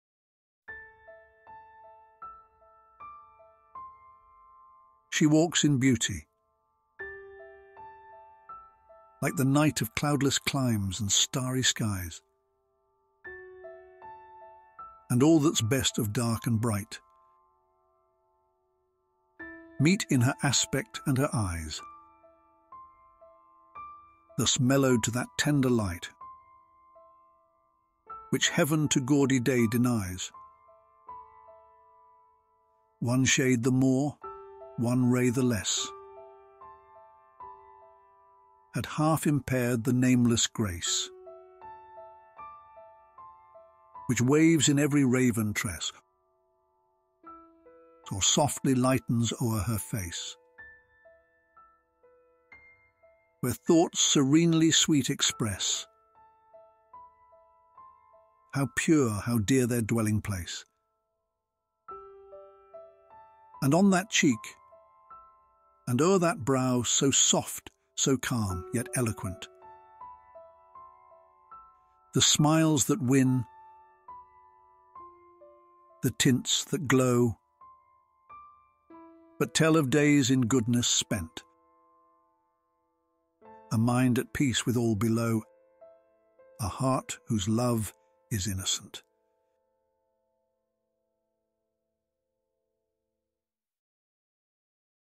She Walks in Beauty by Lord Byron 🎬 4K Cinematic Edit
Lord Byron's "She Walks in Beauty" unveils the exquisite harmony of inner grace and outward beauty, a poetic ode to the captivating essence of femininity. With each stanza, Byron weaves a tapestry of admiration, painting the portrait of a woman whose presence illuminates the very essence of beauty's sublime power, inspiring hearts and minds alike to cherish the enchantment of life's fleeting moments. Let these verses be your guide to appreciate the profound depths of beauty, both seen and felt, as you navigate the wondrous journey of existence.